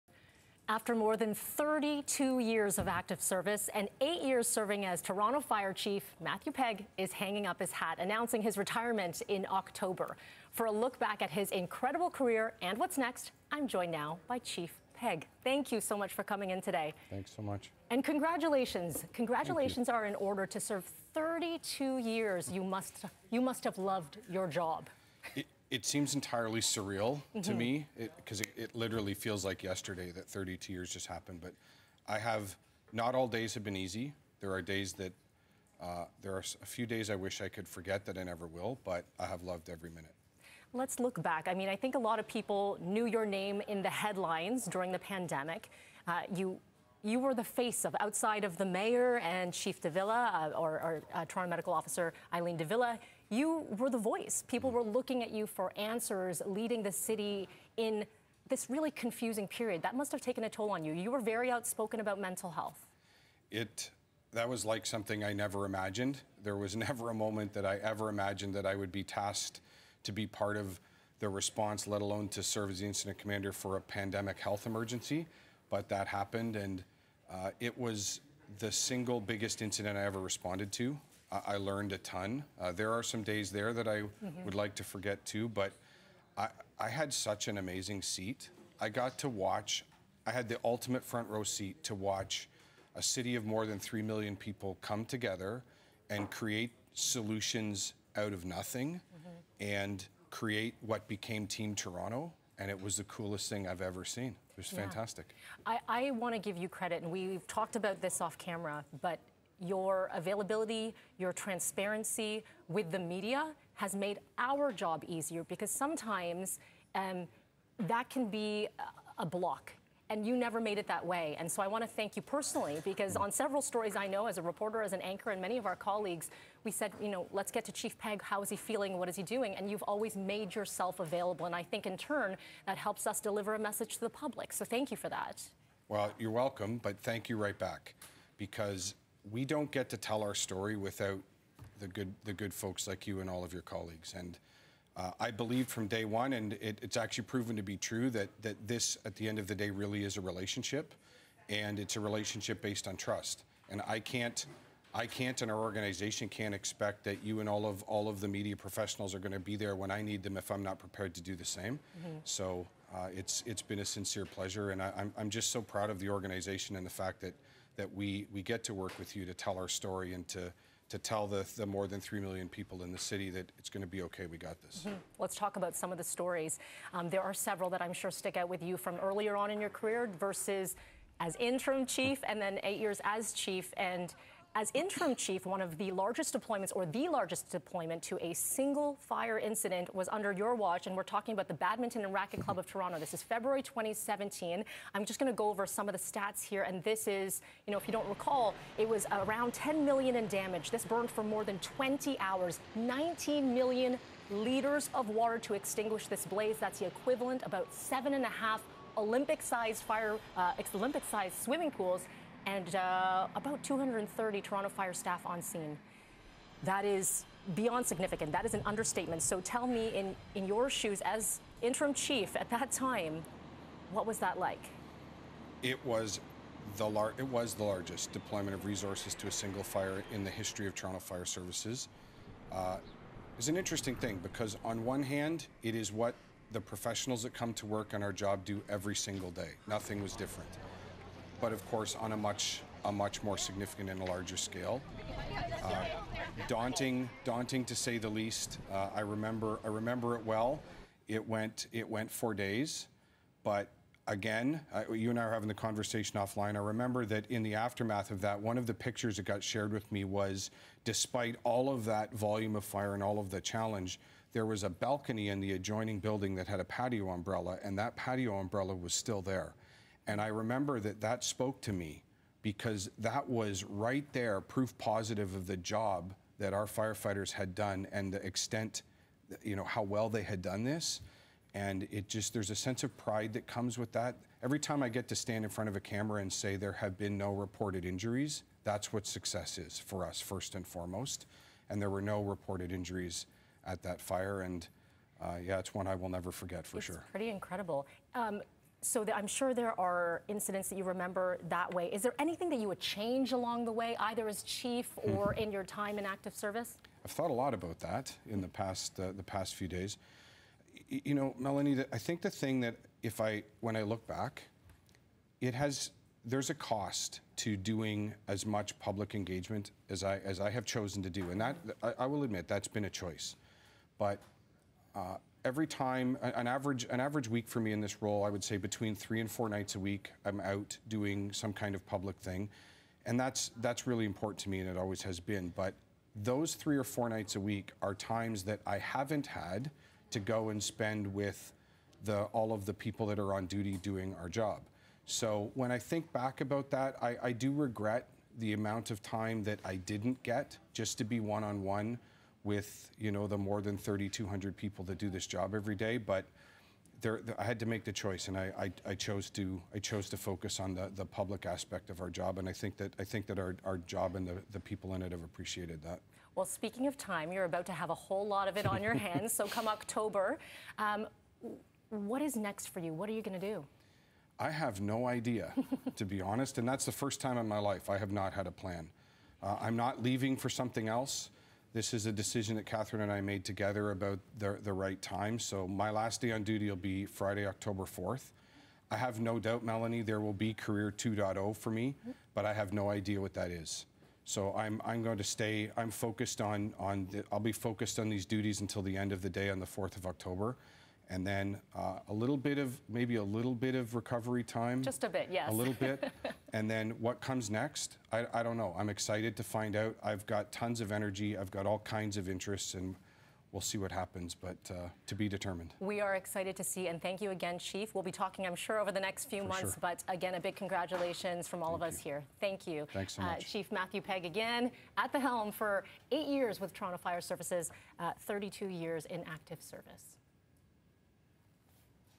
'It seems entirely surreal': Matthew Pegg looks back at his career as Toronto Fire Chief
After more than 32 years of active service, Toronto Fire Chief, Matthew Pegg, is hanging up his hat and will retire in October. Chief Pegg joins Melanie Ng for an interview looking back at his career, and what's next.